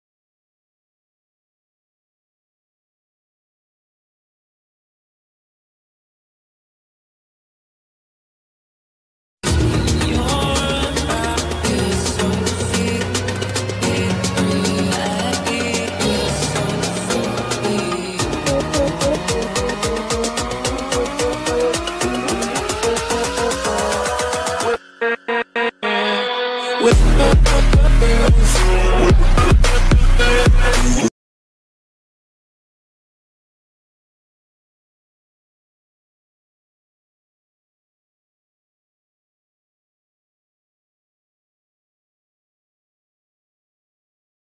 No
I mean no hate to all those people out there who have made a tentacle locker meme like the one i showed if i offended. Anyone with this video then i am sorry i also dont know how to credit yet so ya but if you did like that persons vid then you can try searching up *tentacle locker meme* and it should pop up if you scrowl down a bit